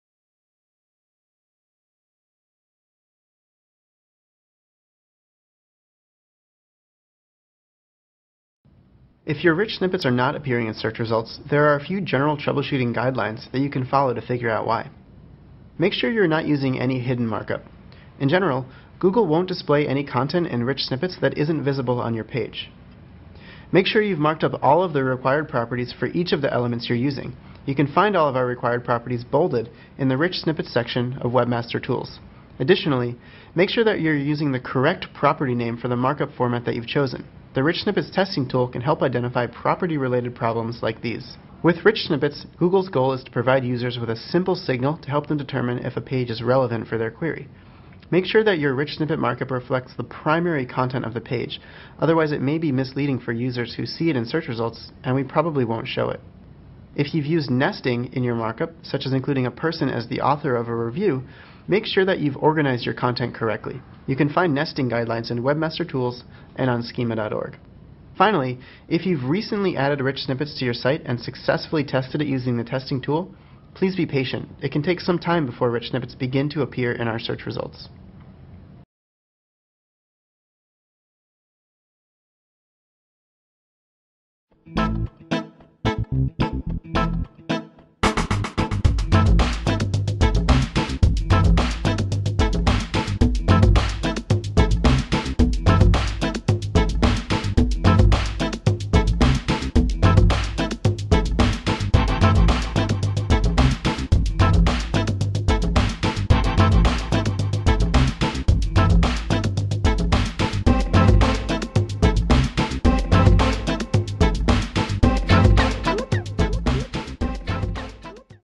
Fragmentos enriquecidos en los resultados de búsqueda | Posicionamiento Web
Los fragmentos enriquecidos otorgan a Google una información adicional sobre el contenido de tu web y facilitan que tanto Google como los usuarios determinen si el contenido que ofreces es relevante a su búsqueda.

Ahora bien, ¿cómo hacemos que nuestros fragmentos enriquecidos aparezcan en los resultados de búsqueda? En este vídeo aprendemos los mejores consejos para conseguirlo y mejorar así nuestro posicionamiento web.

Para empezar, deberás evitar a toda costa los contenido ocultos, ya que Google no lo mostrará. También es importante marcar las propiedades requeridas, detalladas dentro del apartado de Fragmentos enriquecidos en Herramientas para webmasters y tener en cuenta el formato del marcado.

También es importante considerar los anidamientos y la relevancia de los fragmentos con el contenido de tu web. De nuevo, el usuario es el objetivo primordial de Google, y el que tendrá que primar si queremos mejorar nuestro posicionamiento web.

Además, Google pone a tu disposición la Herramienta de pruebas, para comprobar si has añadido correctamente tus fragmentos enriquecidos.

Si aún tenéis dudas sobre cómo hacer que aparezcan tus fragmentos enriquecidos en los resultados de búsqueda, o queréis mejorar vuestro posicionamiento web, ponéos en contacto con nosotros a través de los comentarios de este vídeo.